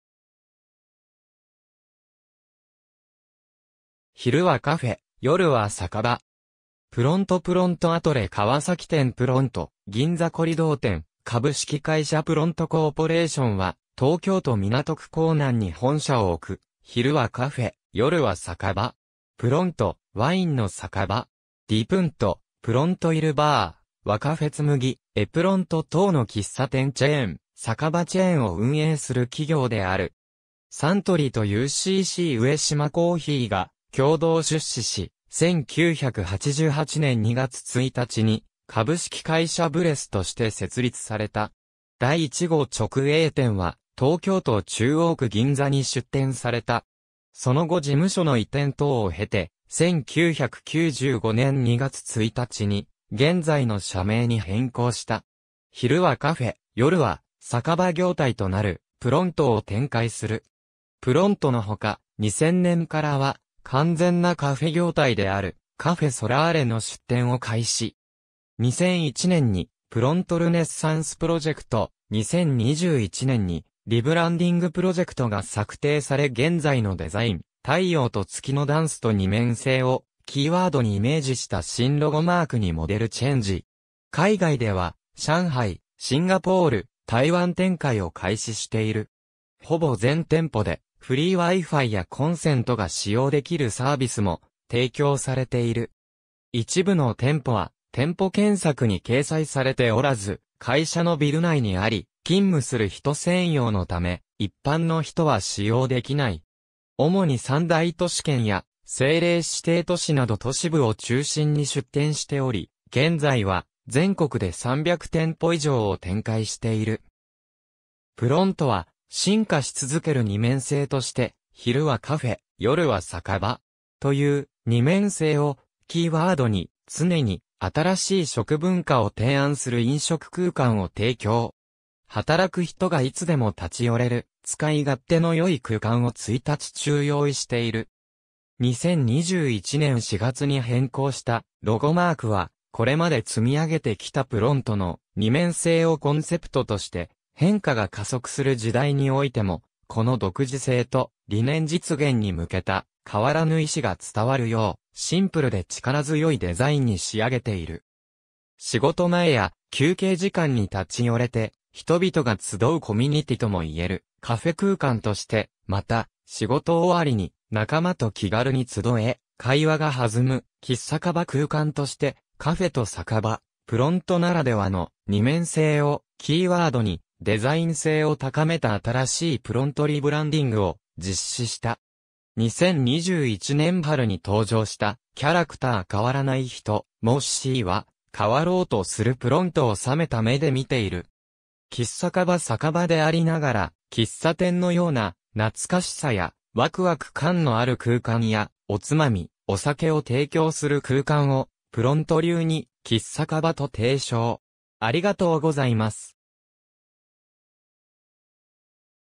プロントコーポレーション
昼はカフェ、夜はサカバ。
PRONTO（プロント） PRONTO アトレ川崎店 PRONTO 銀座コリドー店 株式会社プロントコーポレーション （PRONTO Corporation） は、東京都港区港南に本社を置く、「昼はカフェ、夜はサカバ。PRONTO（プロント）」・「ワインの酒場。ディプント」・「PRONTO IL BAR（イルバール）」・「和カフェ Tsumugi（ツムギ）」・「エプロント」等の喫茶店チェーン、酒場チェーンを運営する企業である。
サントリーとUCC上島珈琲が共同出資し、1988年2月1日に「株式会社ブレス」として設立された。
第1号直営店は東京都中央区銀座に出店された。
その後事務所の移転等を経て、1995年2月1日に現在の社名に変更した。
昼はカフェ、夜は酒場業態となる「PRONTO（プロント）」を展開する。
「PRONTO（プロント）」のほか、2000年からは、完全なカフェ業態である「CAFE SOLARE」の出店を開始。
2001年に「プロントルネッサンスプロジェクト」、2021年に「リブランディングプロジェクト」が策定され現在のデザイン「太陽と月のダンス」と二面性をキーワードにイメージした新ロゴマークにモデルチェンジ。
海外では、上海、シンガポール、台湾展開を開始している。
ほぼ全店舗で、フリーWi-Fiやコンセントが使用できるサービスも提供されている。
一部の店舗は、店舗検索に掲載されておらず、会社のビル内にあり、勤務する人専用のため、一般の人は使用できない。
主に三大都市圏や政令指定都市など都市部を中心に出店しており、現在は全国で300店舗以上を展開している。
（海外含む、2021年12月時点） プロントは、進化し続ける二面性として、「昼はカフェ、夜はサカバ。」という「二面性」をキーワードに、 常に新しい食文化を提案する飲食空間を提供。
働く人がいつでも立ち寄れる、使い勝手の良い空間を一日中用意している。
2021年4月に変更したロゴマークは、これまで積み上げてきたプロントの「二面性」をコンセプトとして、変化が加速する時代においても、この独自性と、理念実現に向けた変わらぬ意思が伝わるよう、シンプルで力強いデザインに仕上げている。
仕事前や休憩時間に立ち寄れて、人々が集うコミュニティともいえるカフェ空間として、 また、仕事終わりに仲間と気軽に集え、会話が弾むキッサカバ空間（※）として、カフェとサカバ、プロントならではの「二面性」をキーワードに、デザイン性を高めた新しいプロントリブランディングを実施した。
2021年春に登場したキャラクター変わらないひと「Mossy（もっしー）」は、変わろうとするプロントをさめた目で見ている。
※「キッサカバ（喫茶酒場）」 酒場でありながら、喫茶店のような懐かしさやワクワク感のある空間やおつまみ、お酒を提供する空間をプロント流に「キッサカバ」と提唱。